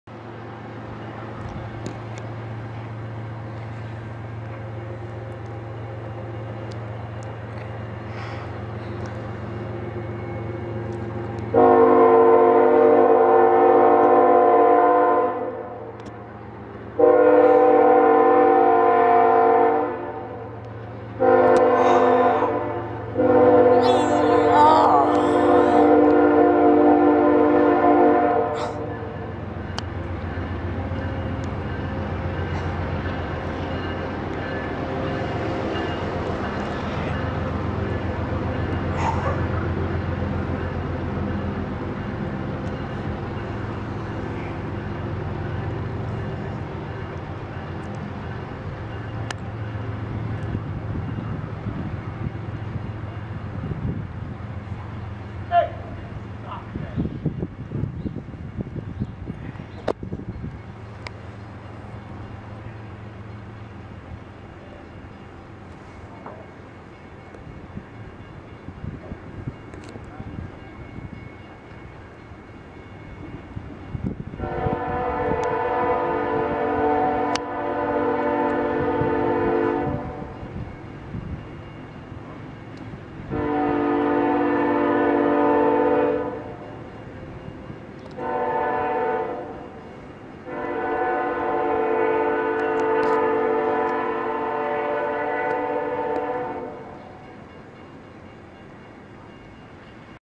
(HD)UP MPRWI w/ GEVO & NS C44-9W trailin in Springfield Illinois
Union Pacific MPRWI (UP/CMO/GATX Grain Train) heads South with a C45ACCTE Leader including dpu.